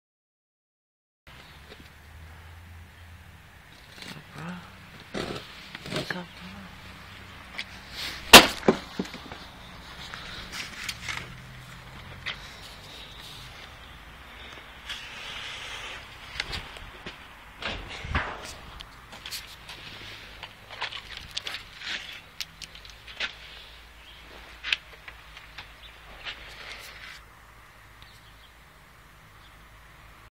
killing santa
decided to kill santa and dispose of the body